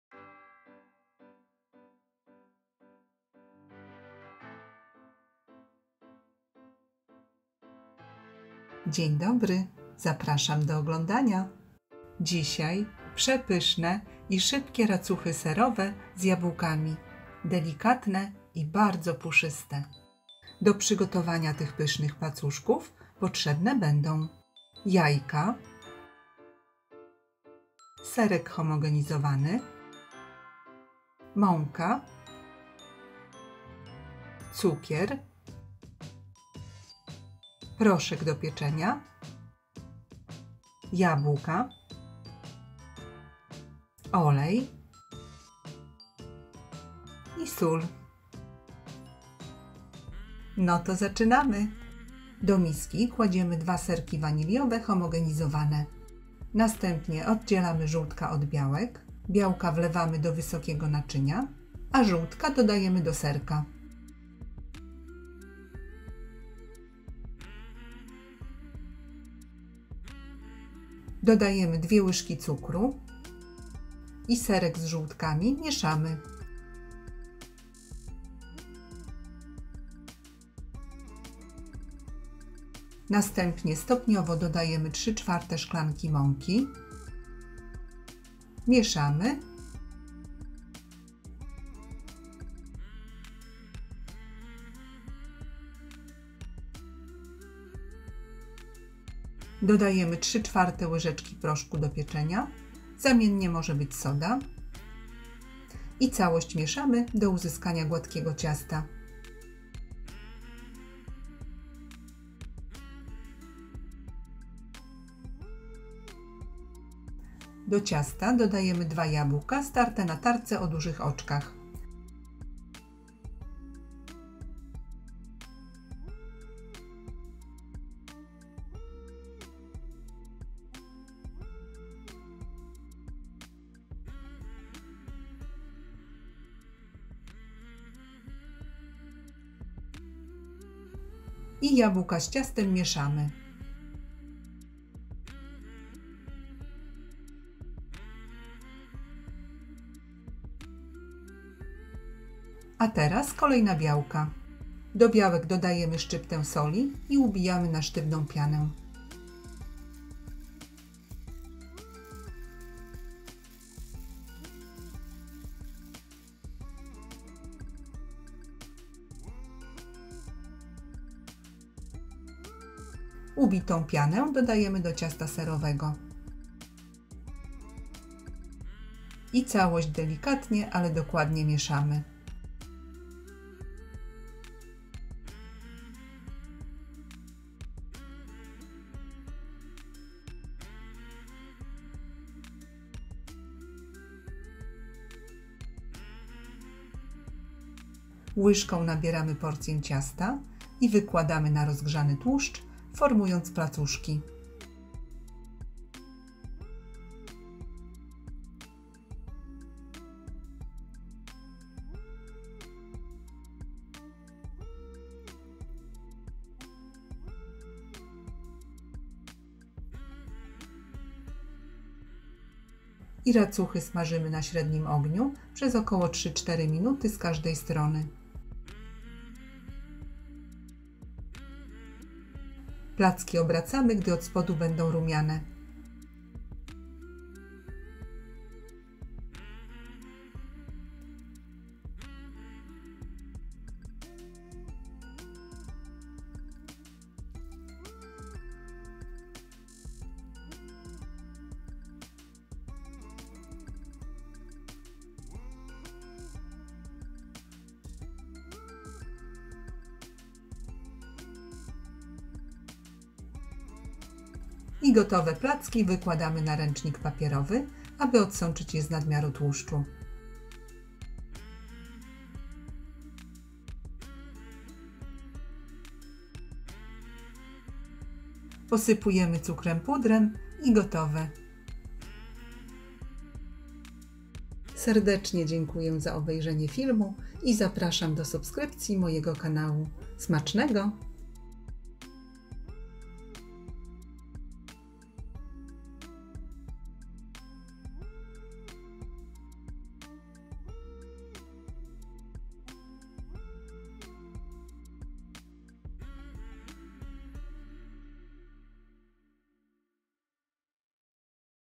Obłędnie pyszne racuchy z jabłkami na serkach homogenizowanych 👌 szybkie i puszyste 👍 rewelacyjne!
Przepyszne, puszyste i mięciutkie racuchy z jabłkami bez drożdży, na kolację lub śniadanie 😍 na serkach homogenizowanych 😋 coś pysznego, najlepsze!
Dokładna lista składników na początku i na końcu filmu 🙂
Modyfikować i zamieniać składniki oczywiście można ale na własne ryzyko 😉

SKŁADNIKI:
2 jajka (112 g)
2 serki homogenizowane waniliowe (2x150 g)
3/4 szklanki mąki (120 g)
2 łyżki cukru (25 g)
3/4 łyżeczki proszku do pieczenia (3 g)
2 jabłka (300 g)
olej do smażenia
szczypta soli